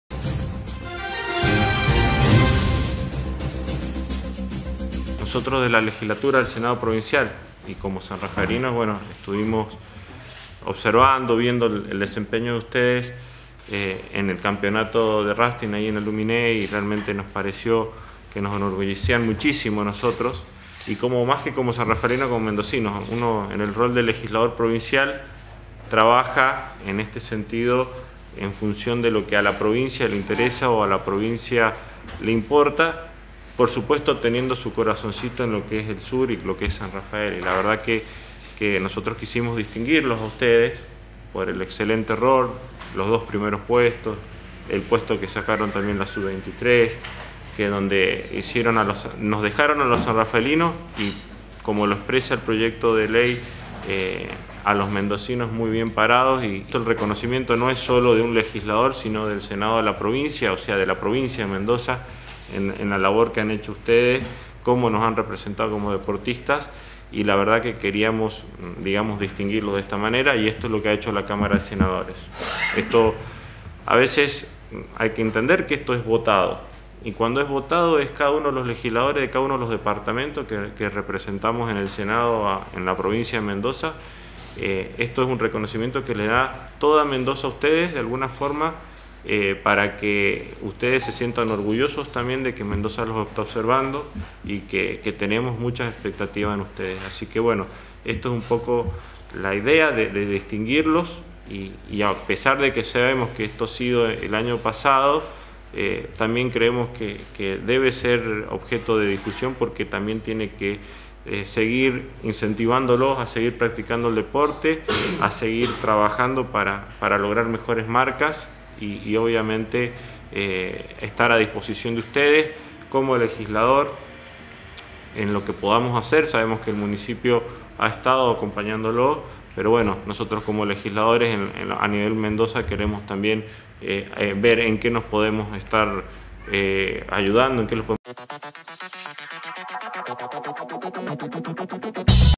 RECONOCIMIENTO LEGISLATIVO
El senador Samuel Barcudi impulsó una iniciativa en la Legislatura de Mendoza para reconocer el trabajo y los premios obtenidos a nivel mundial en la disciplina del rafting.